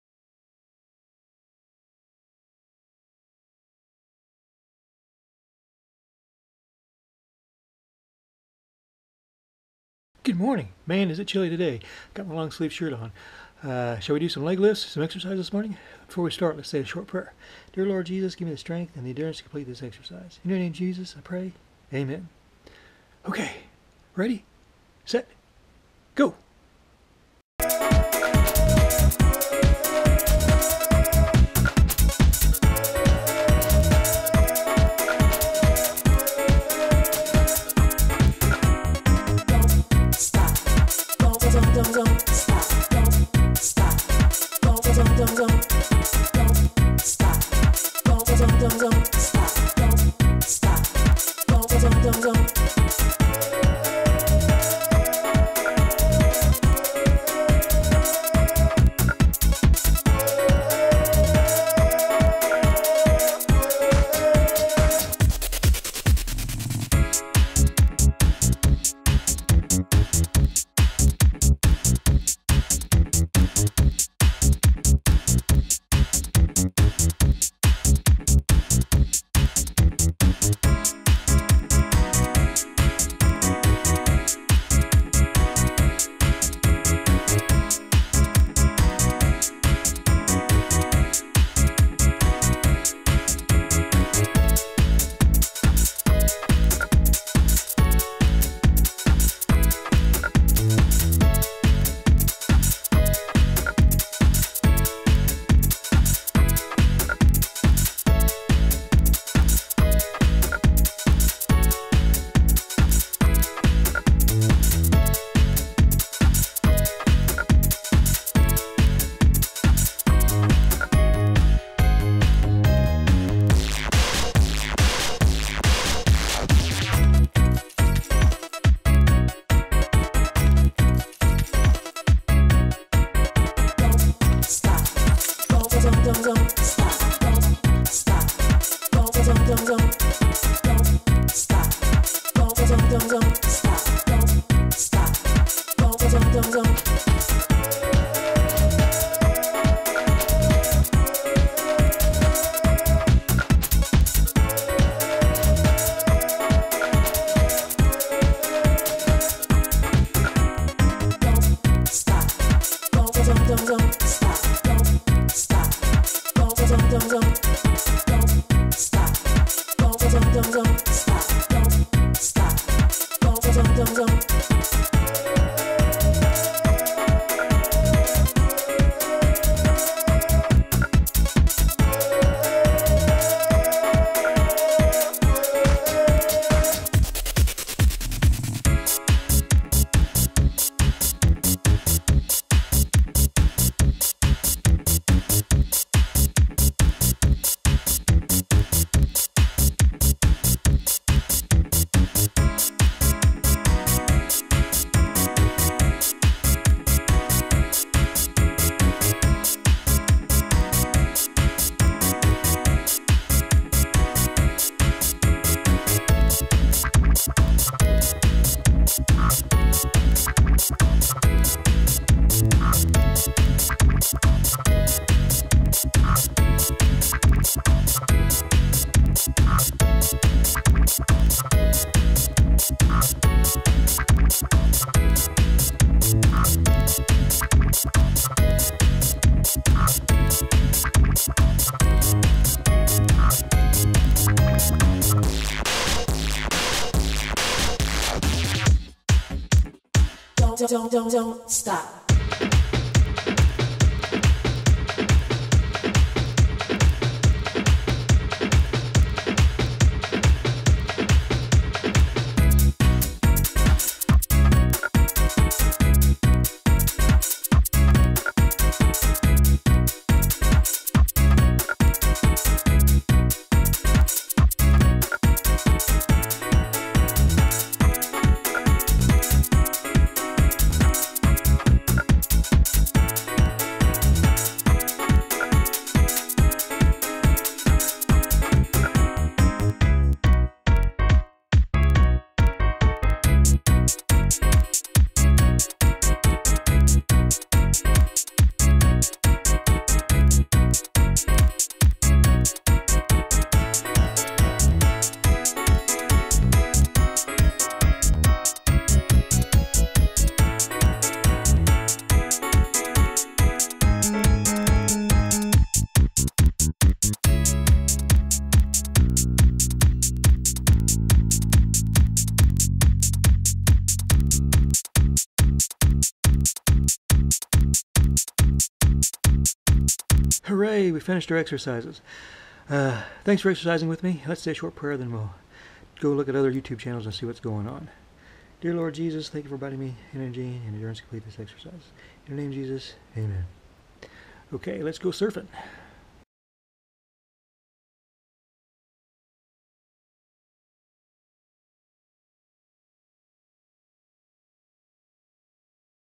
Leg Lifts
Good morning, Ready to do Leg Lifts exercises for 5 minutes? Ready, Set. GO!